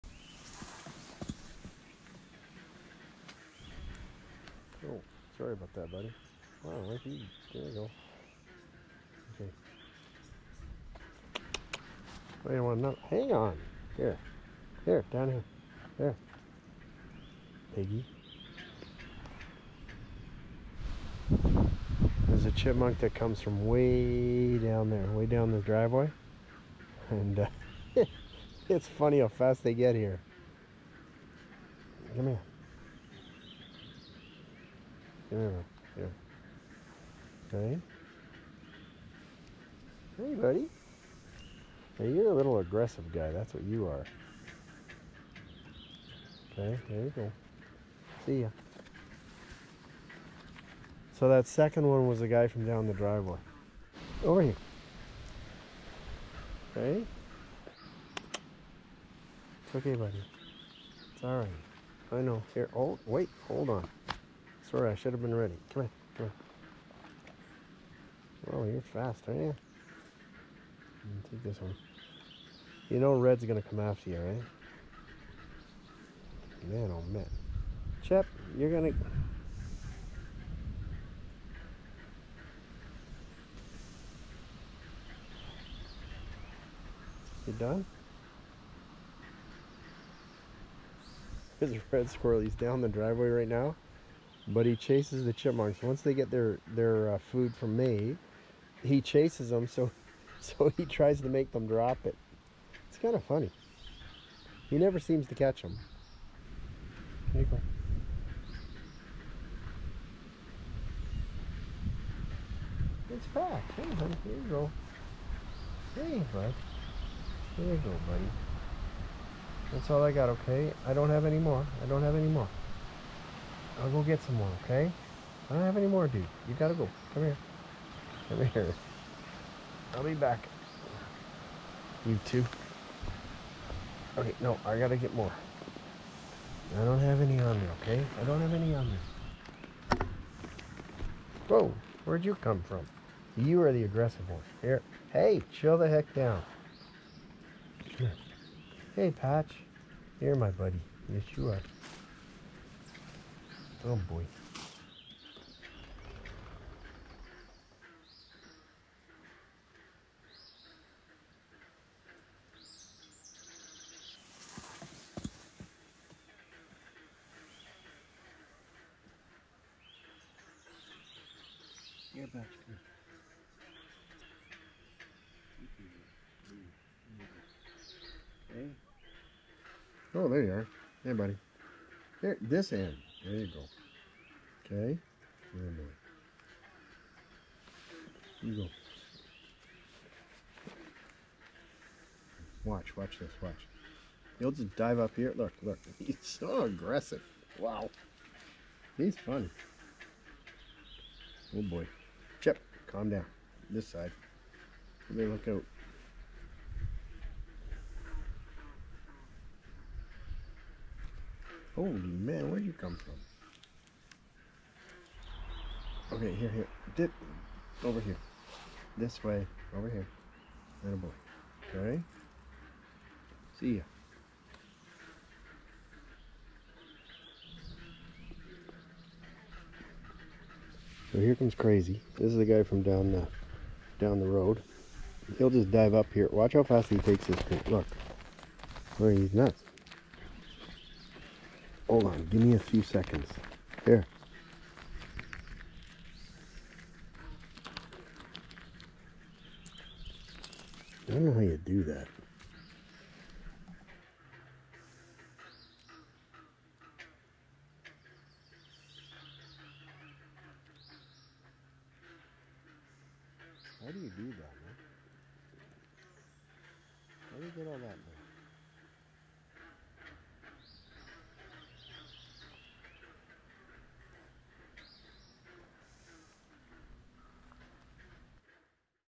Chipmunks Come For A Snack
Several Chipmunks have become attached to me due to the peanuts I carry in my pockets.
Three in particular are so fixated on my every move around the farm that I can't hide from them.  Chipmunks are fun!